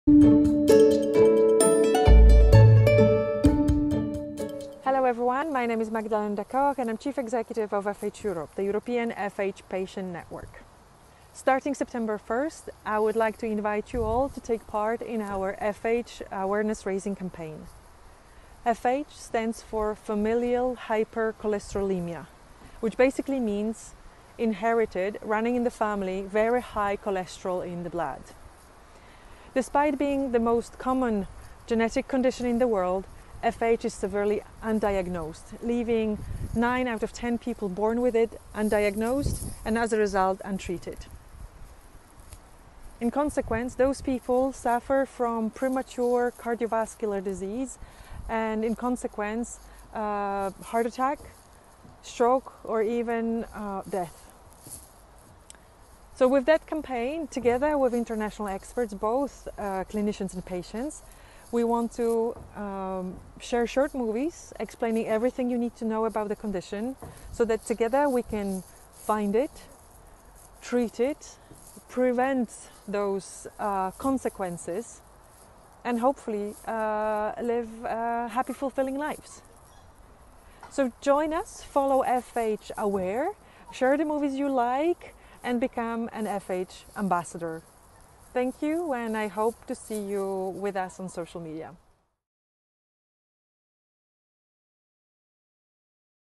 Become an FH Ambassador!
FH stands for Familial Hyper - cholesterol - eamia - inherited, very high cholesterol in the blood. It is estimated that over 30mln people around the world are born with this non-modifiable cardiovascular disease risk factor. Yet, only 10% of people are correctly diagnosed and treated to avoid the terrible consequences of FH.

Become an FHAmbassador NOW!
We need YOU to learn about it, help FindFH and treat it as early as possible to save hearts and lives of our loved ones, friends and colleagues.

FH Awareness Day takes place every year on September 24th!